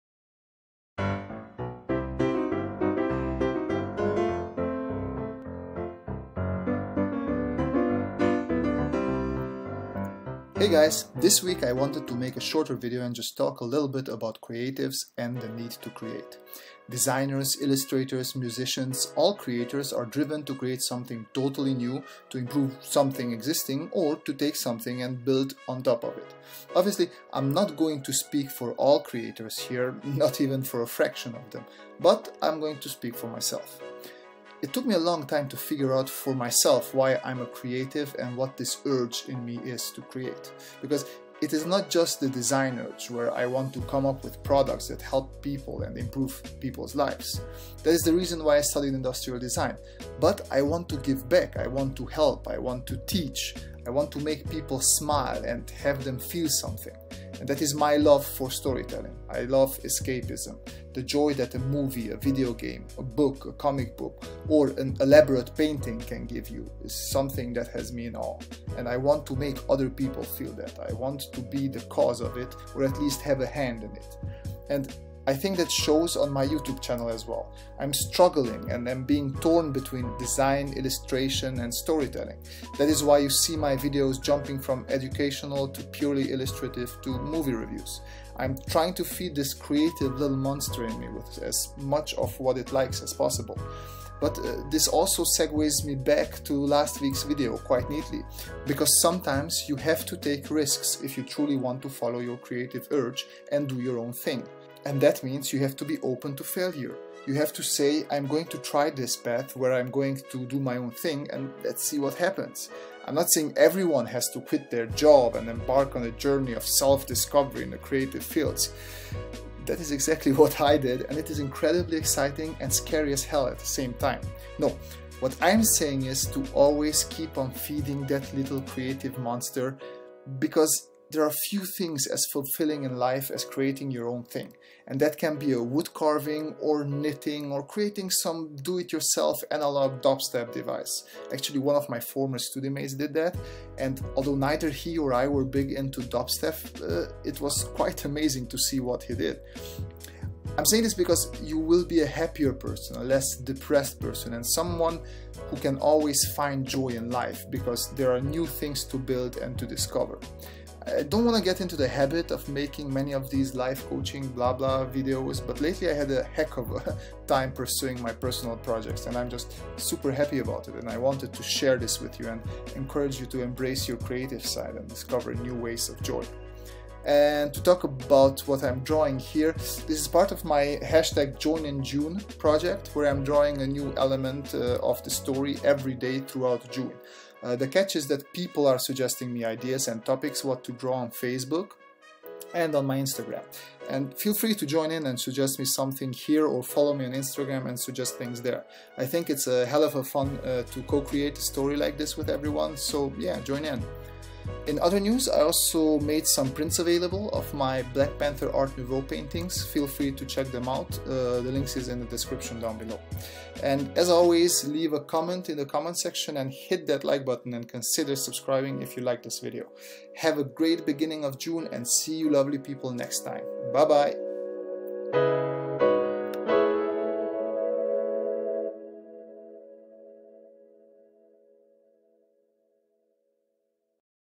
The Need to Create and joininjune
In this one, I talk about the need to create and also about my joininjune project where I draw a piece of an image every day for the month of June, and you kind people can give me daily suggestions here or on my Instagram.

You can get prints of my Black Panther Art Nouveau series

audio